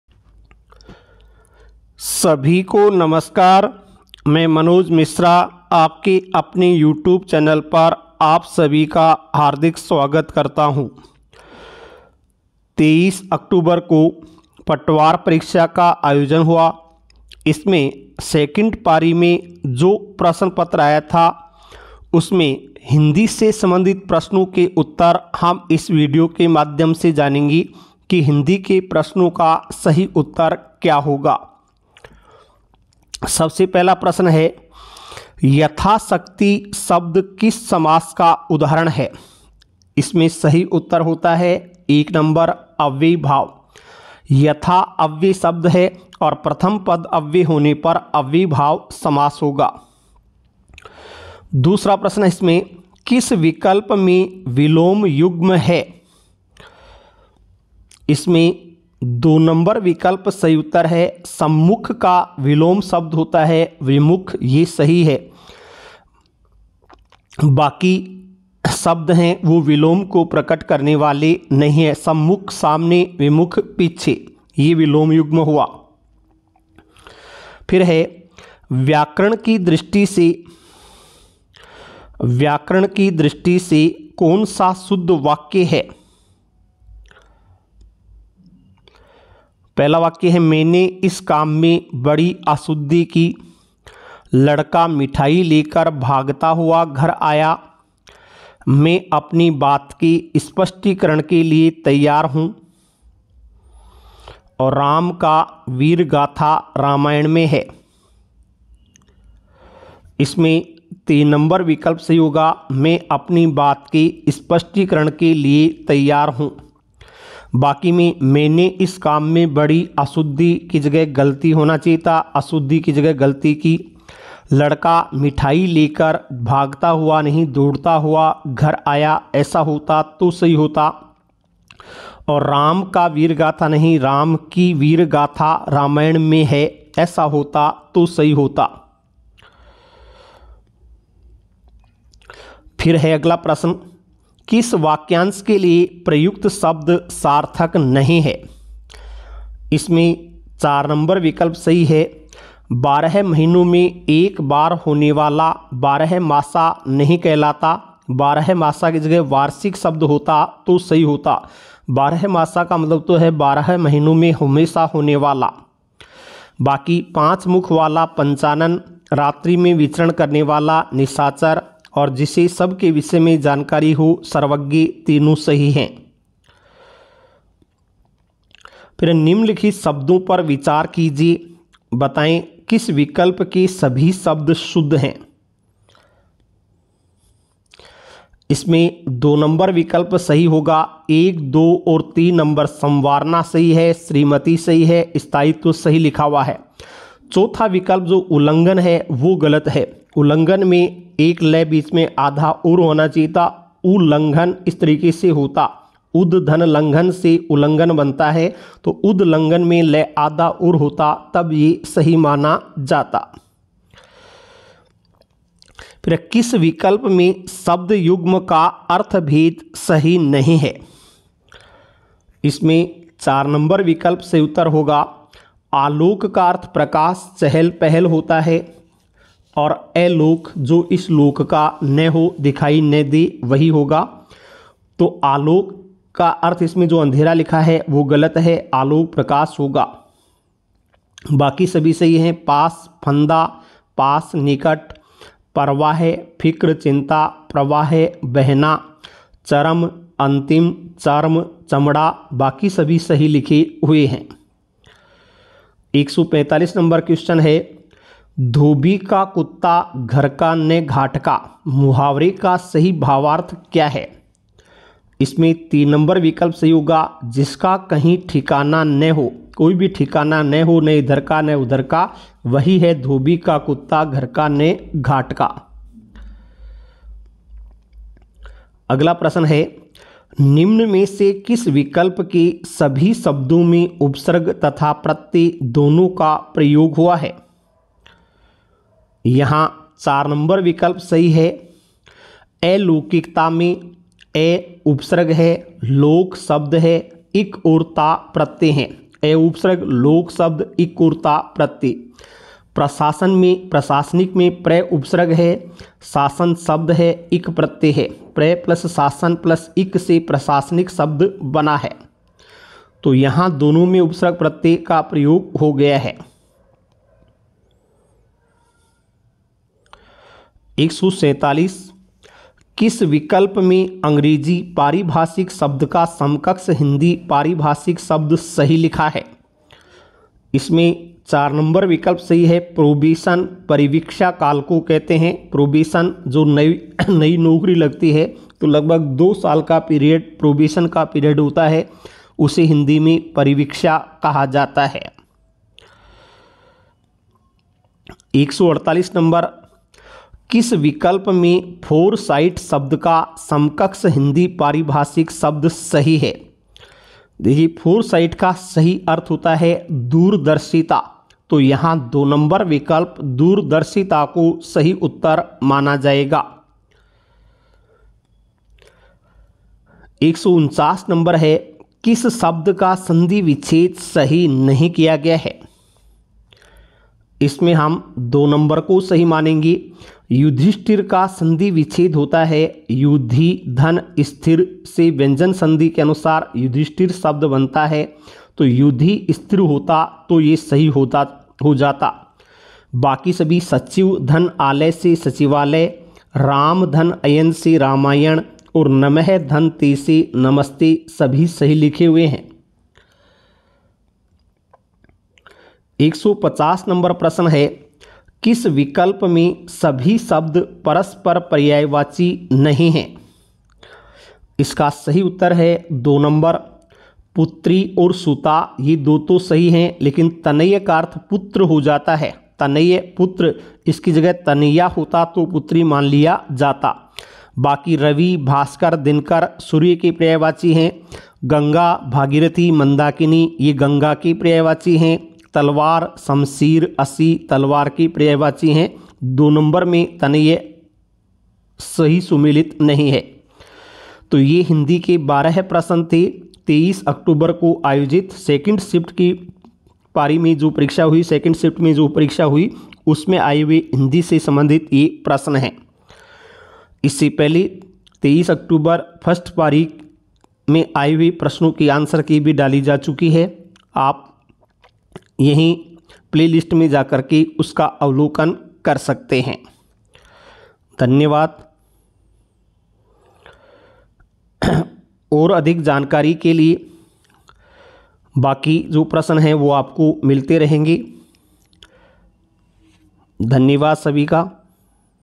Patwar answer key 23 oct 2nd shift Hindi !! By : मनोज मिश्रा परिश्रम सीकर
इस वीडियो में पटवार भर्ती परीक्षा 23 अक्टूबर शाम की पारी में आर्योजित प्रश्न पत्र के हिंदी विषय के प्रश्नों को हल करवाया गया है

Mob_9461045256